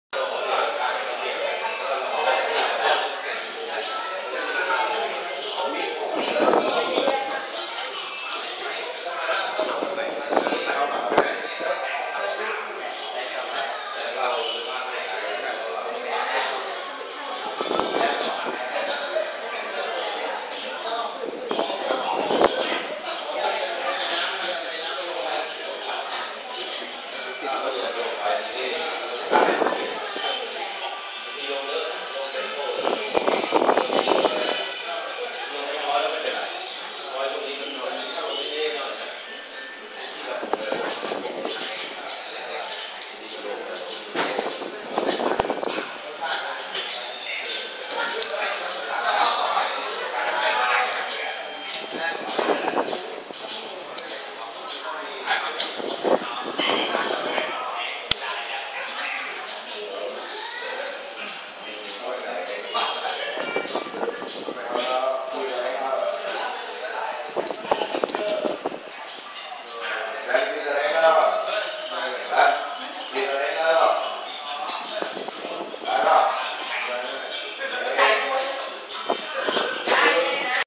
2012-08-13 สงกรานต์ คึ๊ก
วันนี้ไปเผาศพย่า ที่วัดขุนพิทักษ์ จ.ราชบุรี
พาสงกรานต์ไปด้วย เพราะไม่มีใครอยู่บ้าน
เสร็จงานก็เอามาปล่อยให้เดินเล่น ที่บ้านราชบุรี
คึกใหญ่เลย